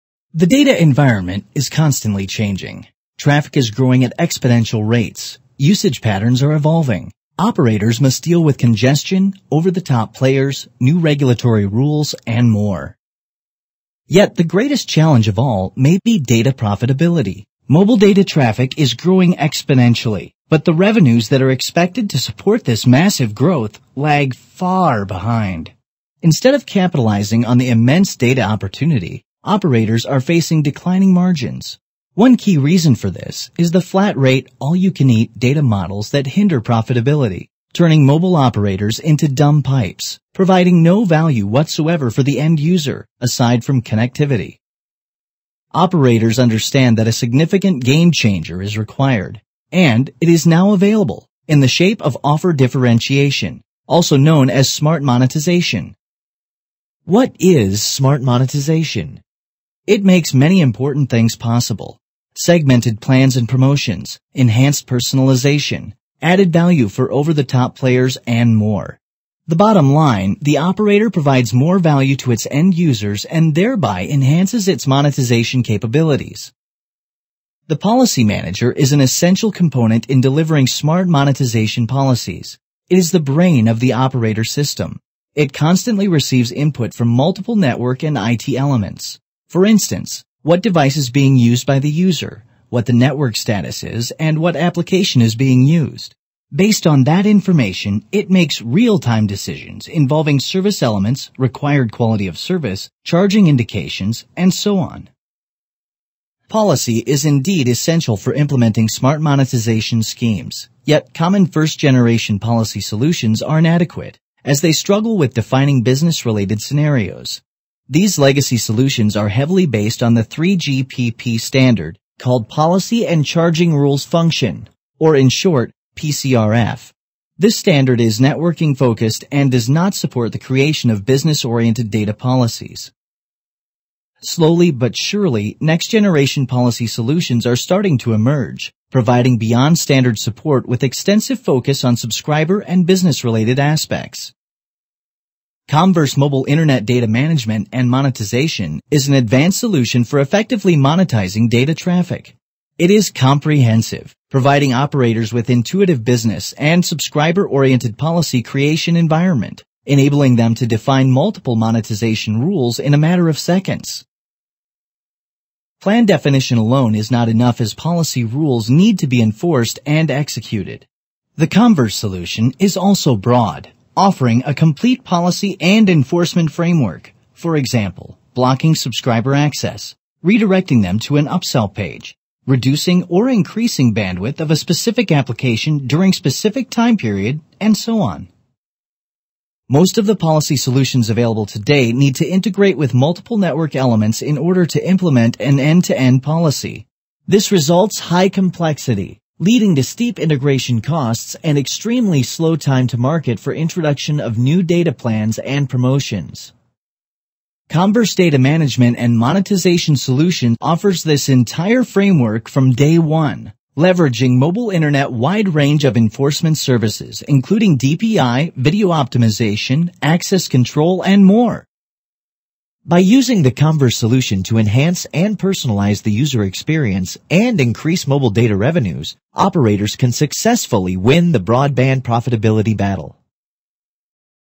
Smart Monetization: Comverse Data Management and Monetization
The data environment is constantly changing. Traffic is growing at exponential rates; usage patterns are evolving. Operators must deal with congestion, over-the-top players, new regulatory rules and more...
Yet the greatest challenge of all may be data profitability. Mobile data traffic is growing exponentially, but the revenues that are expected to support this massive growth lag far behind. Instead of capitalizing on the immense data opportunity, operators are facing declining margins. One key reason for this is the flat-rate "all you can eat" data models that hinder profitability — turning mobile operators into dumb pipes, providing no value whatsoever for the end user, aside from connectivity.
Operators understand that a significant game changer is required. And it is now available: Offer Differentiation -- also known as Smart Monetization.
Smart monetization makes many important things possible: segmented plans and promotions, enhanced personalization, added value for over-the-top players and more. The bottom line: the operator provides more value to its end users and thereby enhances its monetization capabilities.
The Policy Manager is an essential component in delivering smart monetization policies. It is the brain of the operator's system. It constantly receives input from multiple network and IT elements -- for instance: what device is being used by the user, what network status is, what application is being used... Based on that information, it makes real-time decisions involving service entitlements, required quality of service, charging indications and so on.
Policy is essential for implementing smart monetization schemes; yet common 1st-generation policy solutions are inadequate. They struggle with defining business-related scenarios. These legacy solutions are heavily based on the 3GPP standard called Policy and Charging Rules Function (or in short:  PCRF). This standard is network-oriented and does not support the creation of business-oriented data policies.
Emerging next- generation policy solutions can provide beyond-standard support with a focus on subscriber and business related aspects. Comverse Mobile Internet Data Management and Monetization is an advanced solution for effectively monetizing data traffic:
• It is comprehensive — providing operators with intuitive business- and subscriber-oriented policy creation environment, enabling them to define multiple monetization rules in a matter of seconds. Plan definition alone is not enough — policy rules need to be enforced and executed.
• The Comverse solution is also broad, offering a complete policy and enforcement framework -- for example: blocking subscriber access, redirecting them to an up-sell page, reducing  or increasing bandwidth of a specific application during specific time period and so on.
Most of the policy solutions available today need to integrate with multiple network elements in order to implement an end-to-end policy. The complexity results in steep integration costs and extremely slow time to market for new services.
A next-generation solution that pre-integrates this entire framework from Day 1, Comverse Data Management and Monetization leverages mobile Internet's wide range of enforcement services — including DPI, video optimization, access control, and more.
Operators can optimize network performance, enhance and personalize the user experience and maximize data revenues.